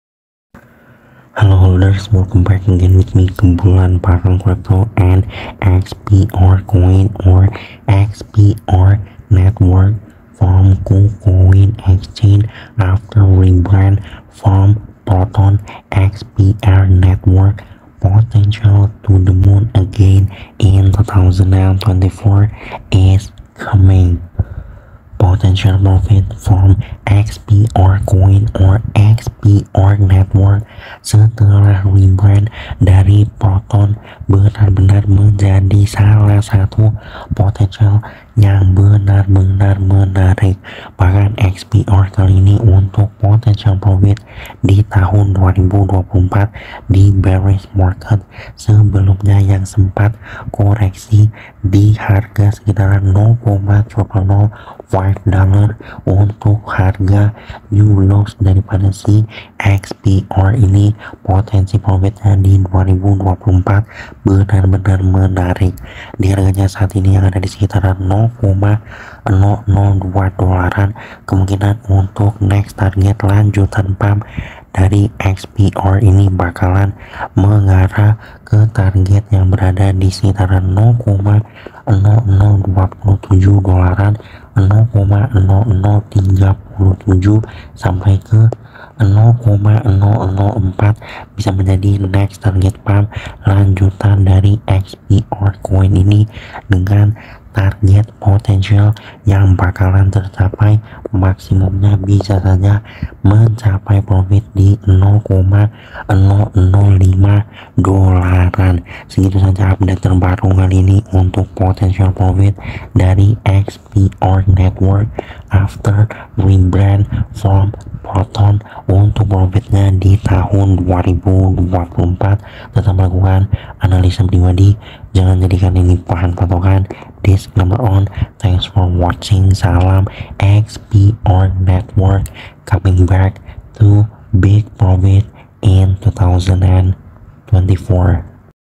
XPR COIN PUMP POTENTIAL IN 2024‼️ XPR NETWORK PRICE PRICE FORECAST‼️ PROTON REBRANDED TO XPR NETWORK
xpr crypto update